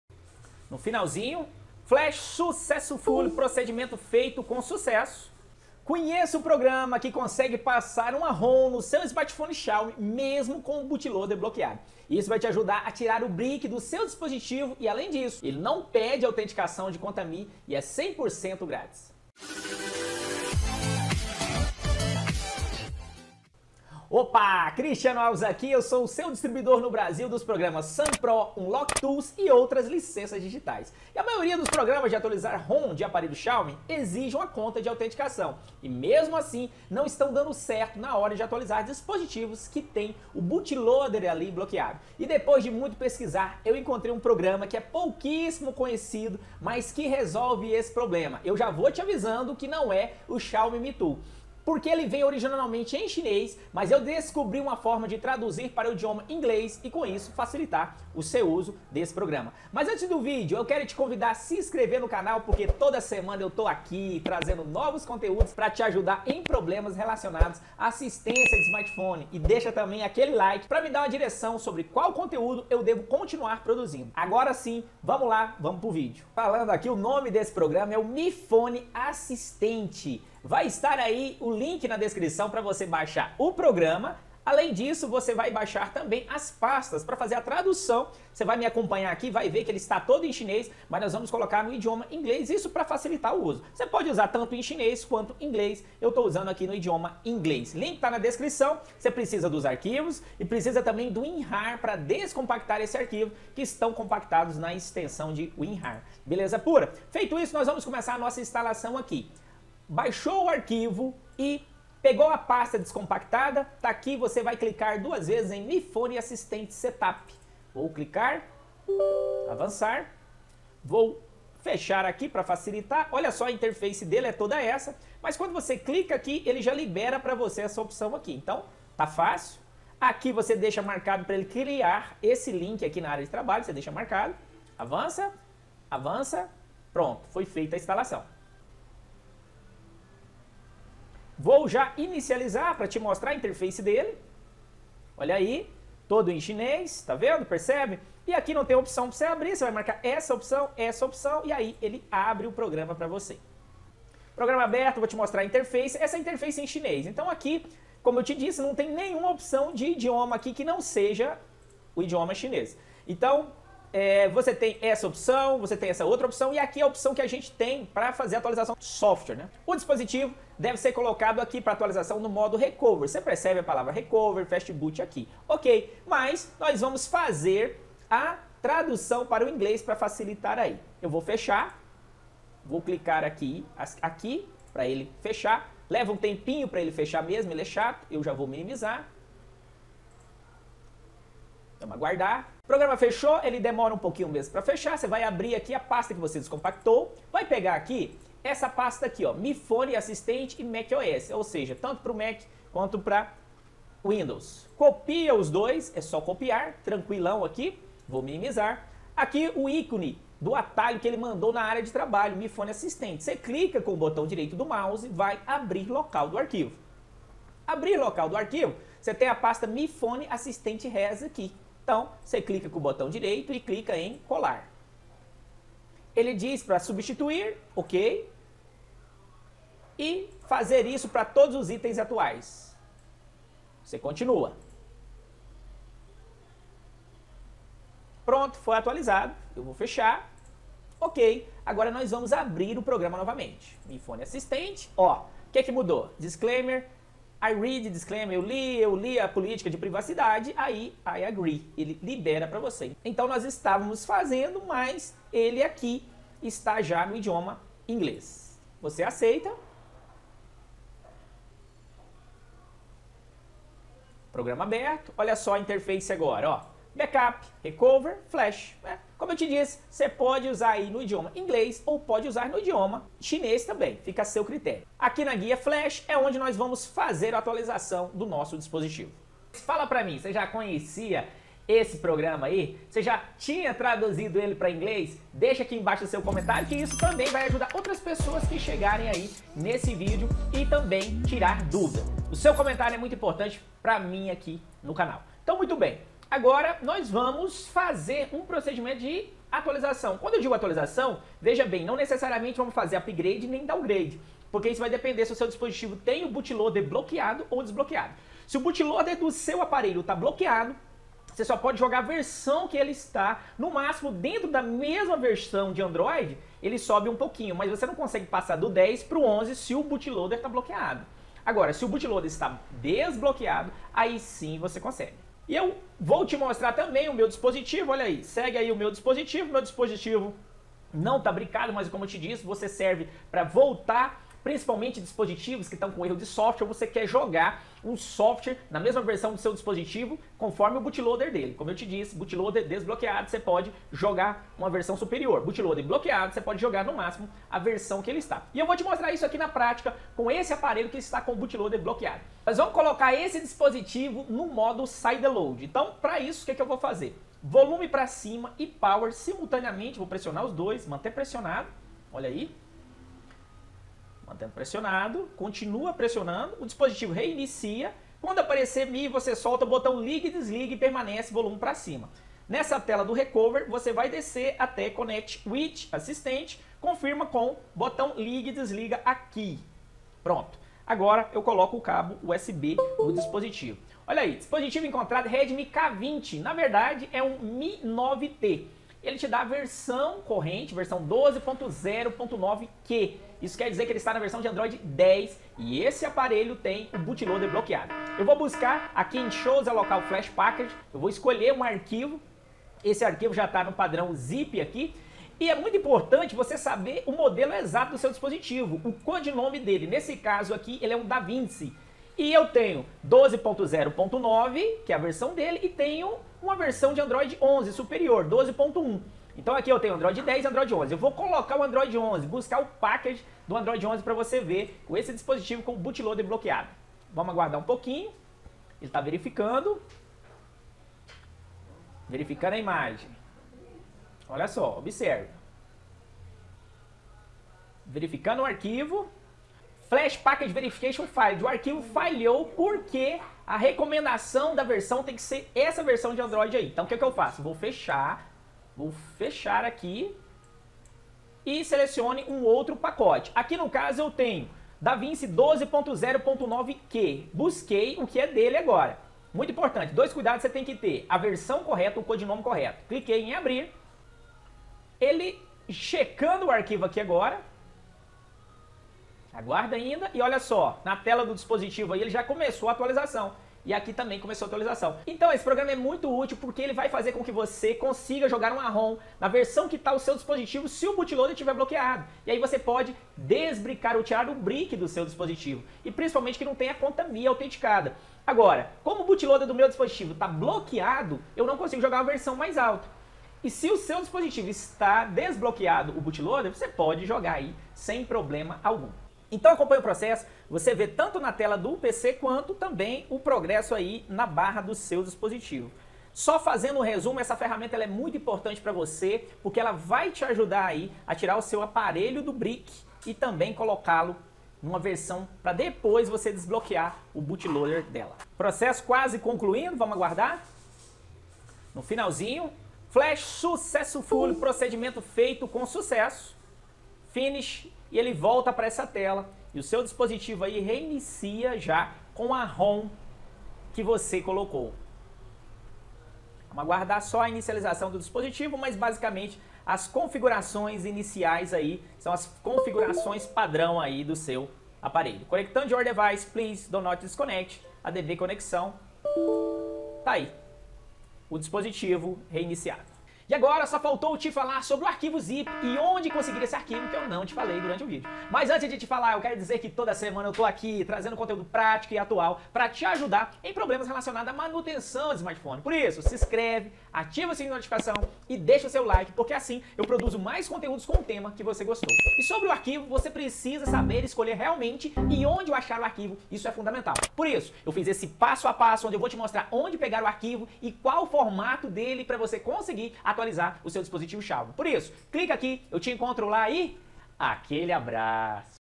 Como Resolver Xiaomi BRICKADO Sem Conta MI Autenticada 💡
Método para recuperar seu Xiaomi Brickado ou atualizar a versão do Android no modo Sideload

⚠️ ISENÇÃO DE RESPONSABILIDADE:
📞 Fale Comigo e Tire Suas Dúvidas:‍